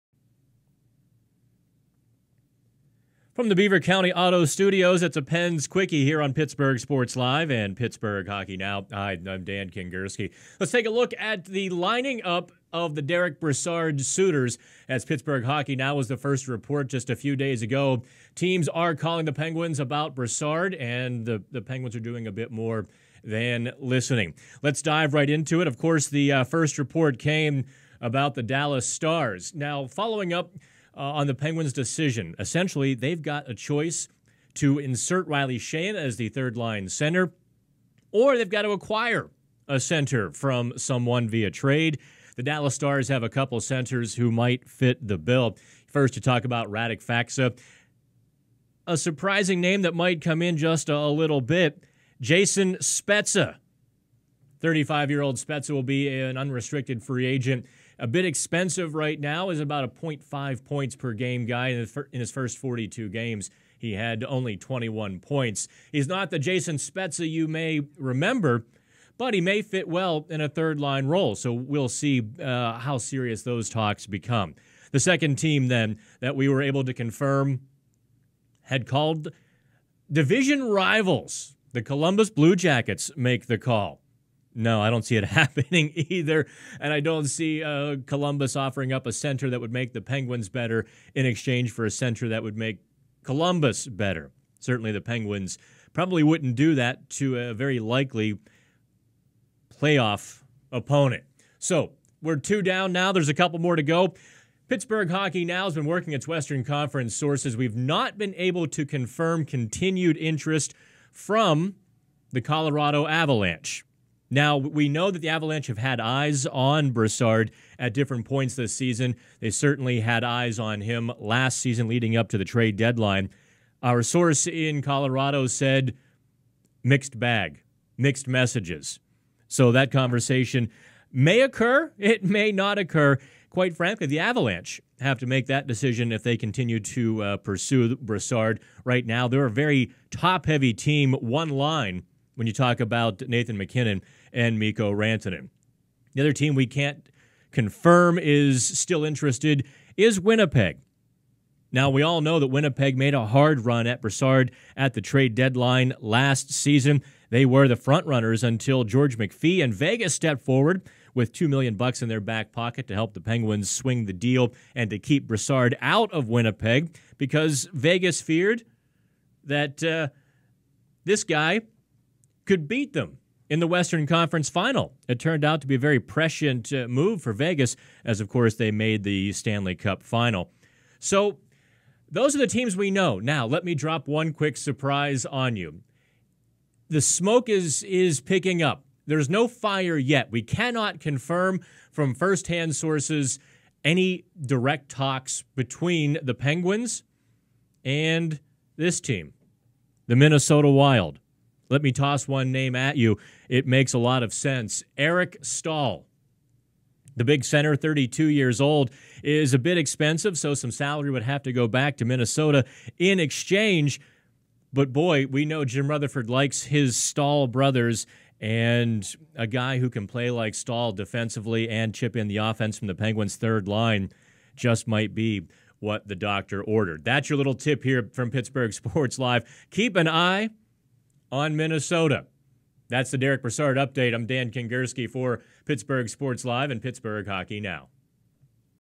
NHL Trade Rumors: Derick Brassard-Penguins Suitors Lining Up | PHN
Pittsburgh Hockey Now reporter Dan Kingerski looks at the teams which are confirmed to have called the Pittsburgh Penguins on Derick Brassard. Dan also drops one surprise team to watch.